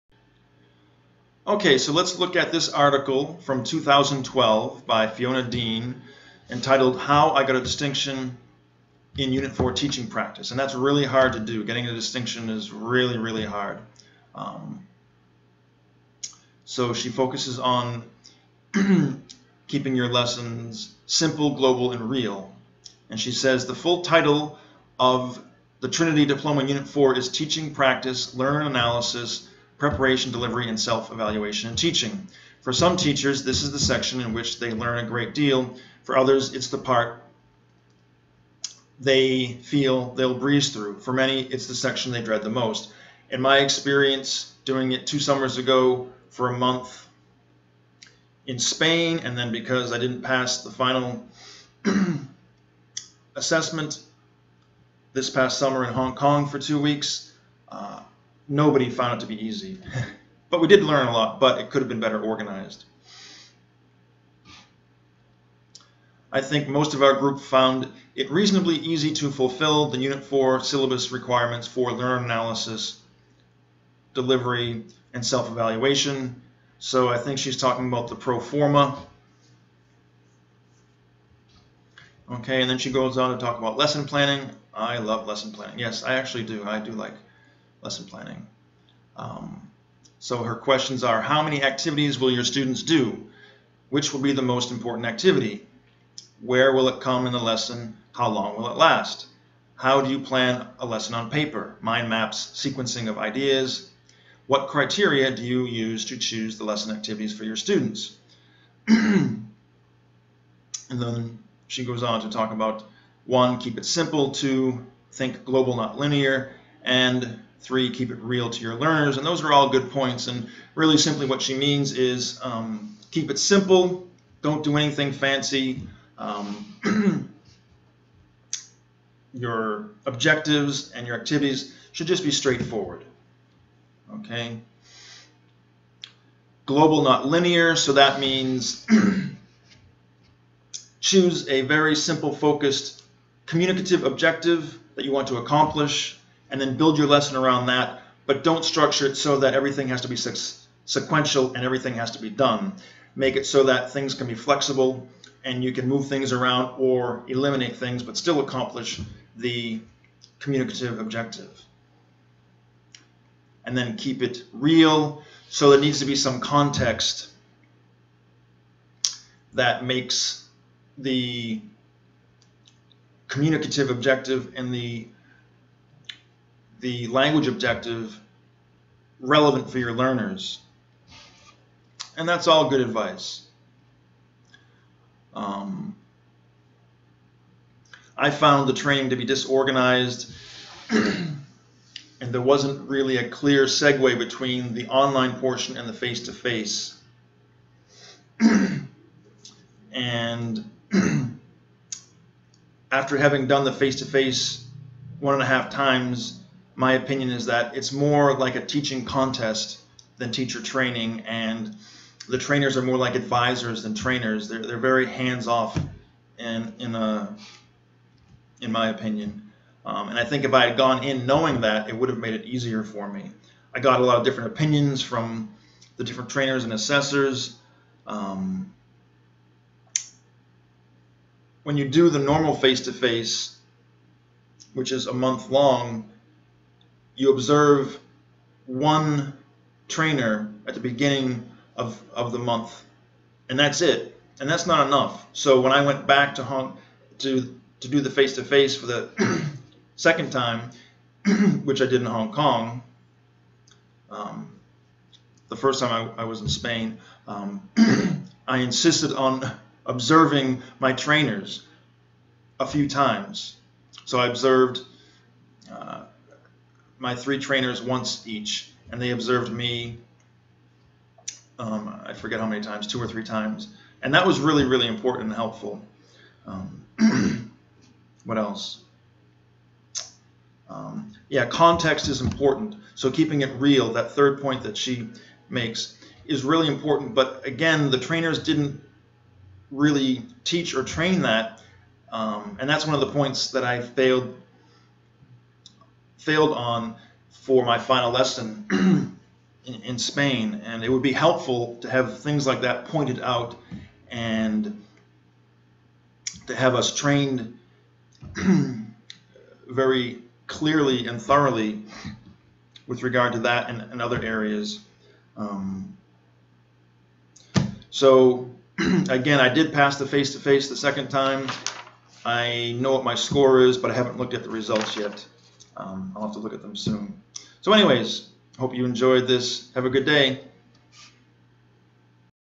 How to Get a Distinction on Trinity Diploma Teaching Module
Fiona Deane: "How I got a distinction in Unit 4 Teaching Practice" and My Experience

This is a review of an article about how a Trinity candidate got a distinction in the Face-to-Face and my experience doing the F2F 1.5 times.